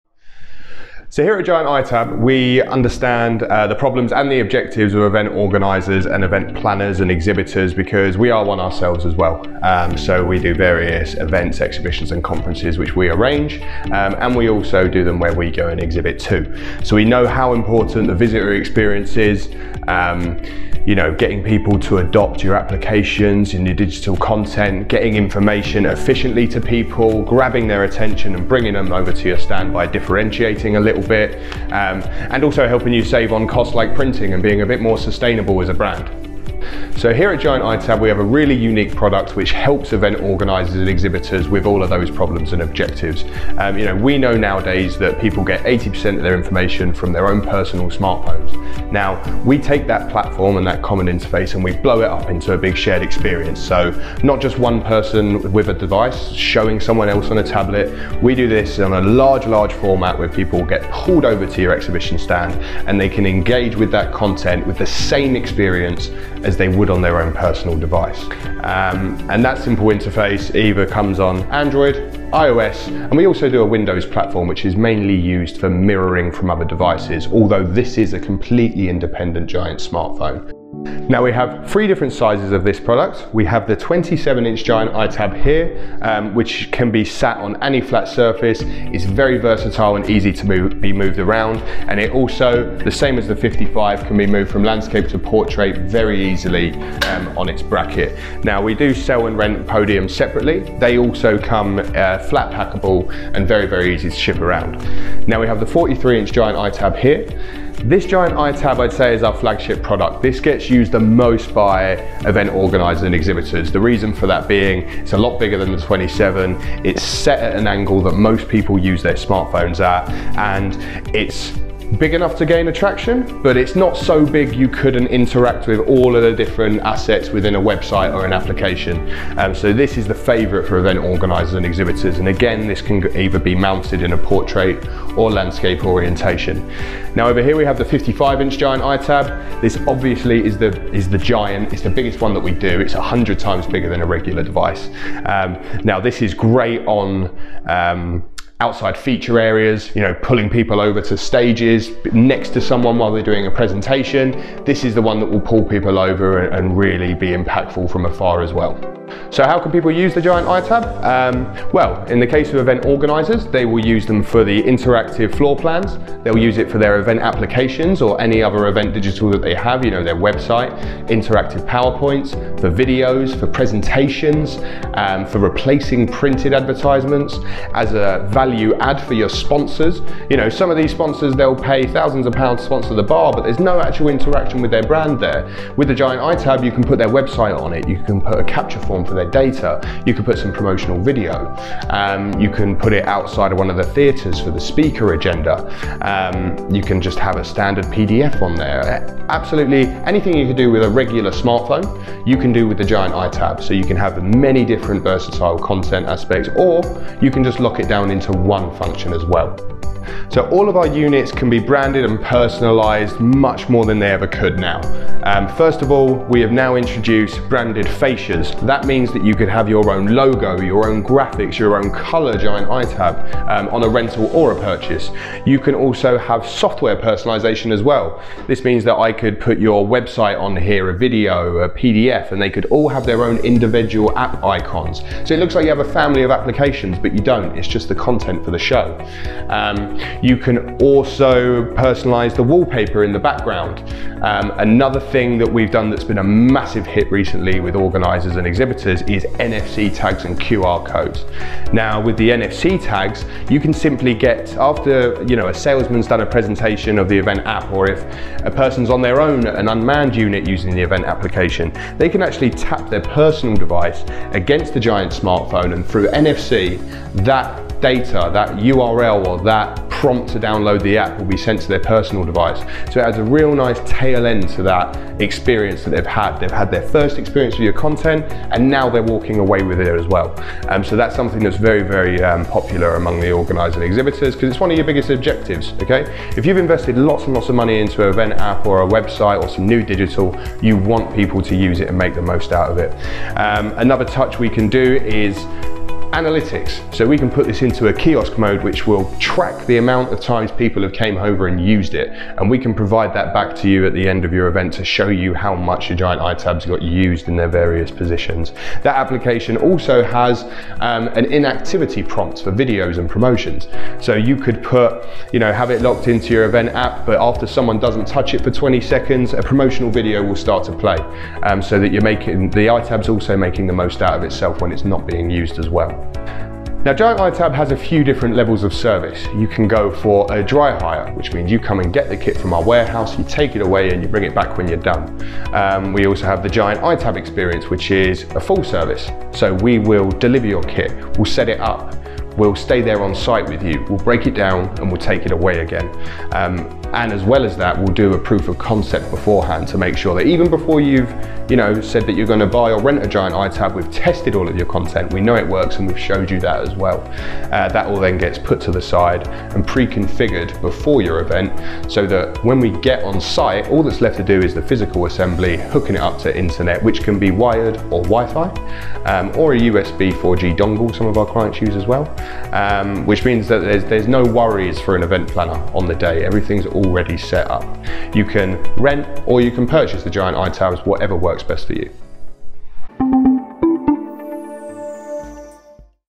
Giant iTab - Everything You Need To Know
Jon from Giant iTab takes you through everything you need to know about our Giant Smartphone Solution.

Giant iTabs are perfect for event planners and event professionals.